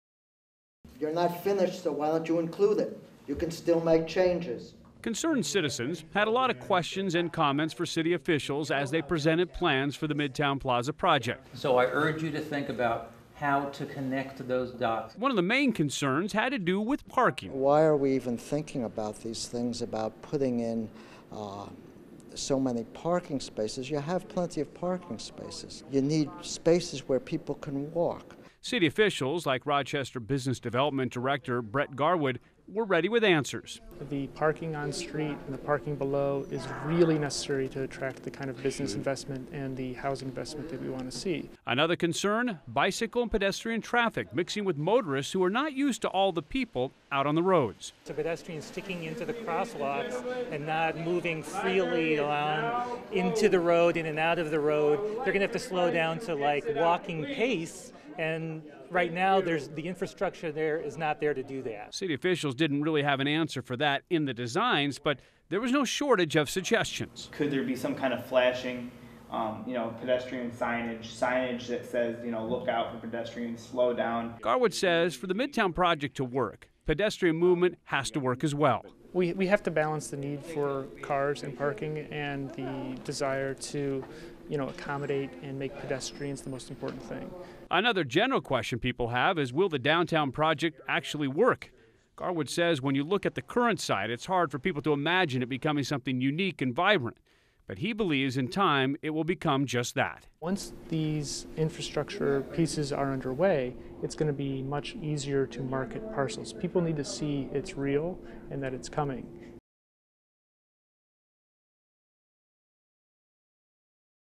Officials present Midtown Plaza plans to public
Rochester City Officials hope in the near future that Midtown Plaza will create a new era for downtown Rochester. On Thursday, they showed the public the latest plans for the plaza's open spaces, and parks.&nbsp;&nbsp; &nbsp;Concerned citizens had a lot of questions and comments for those officials.